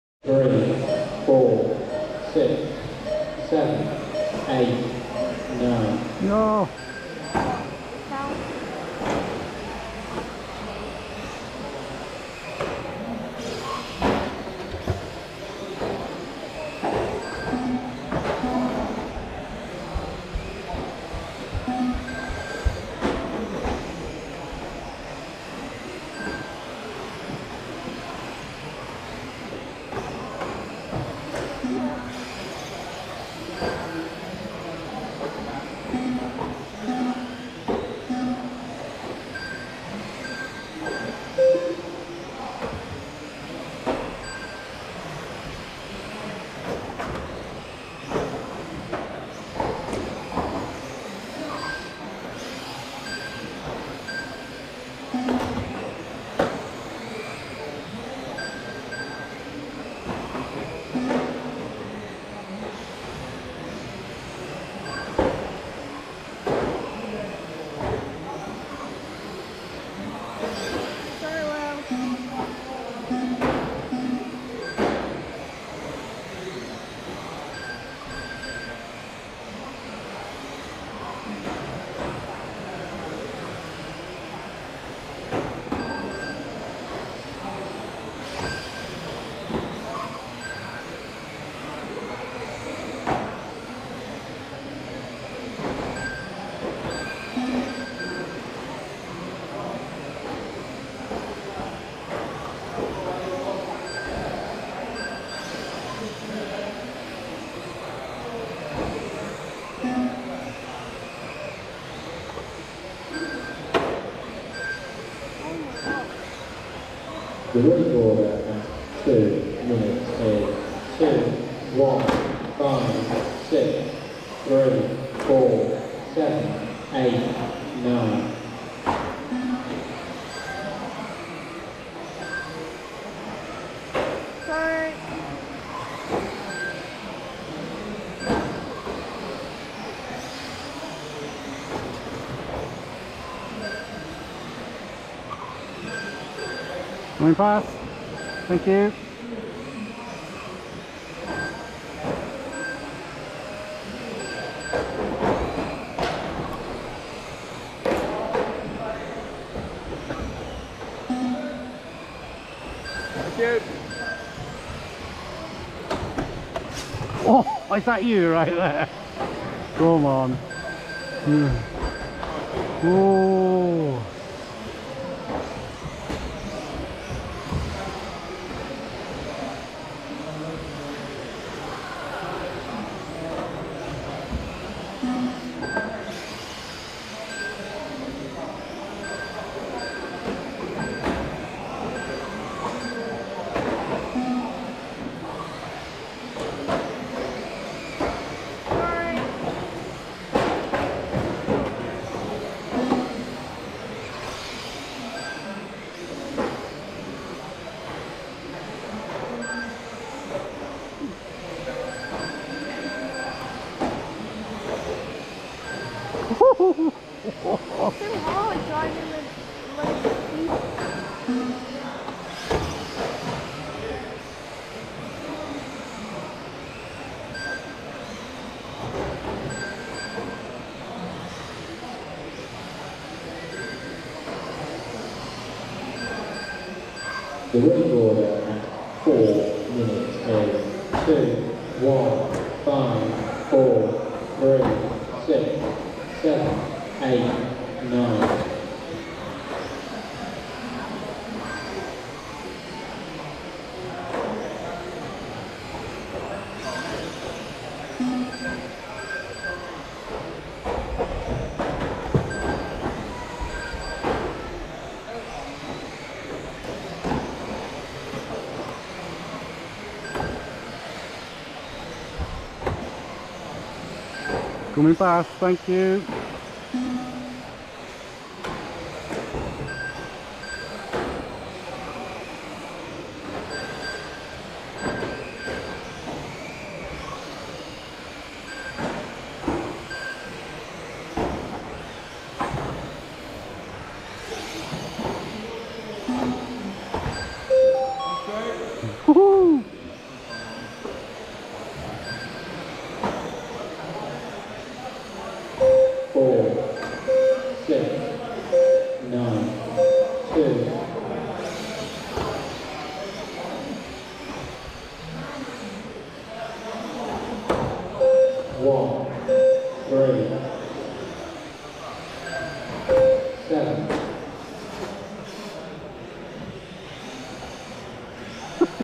WBMCC Winter 24 R4, M-Chassis, Round 3
Another busy night in West Bridgeford.  Two heats of M Chassis and some good racing amongst in the slower heat.  My third heat was my fastest with a few good battles.  I did 18 laps and my second lap was my fastest at 16.06.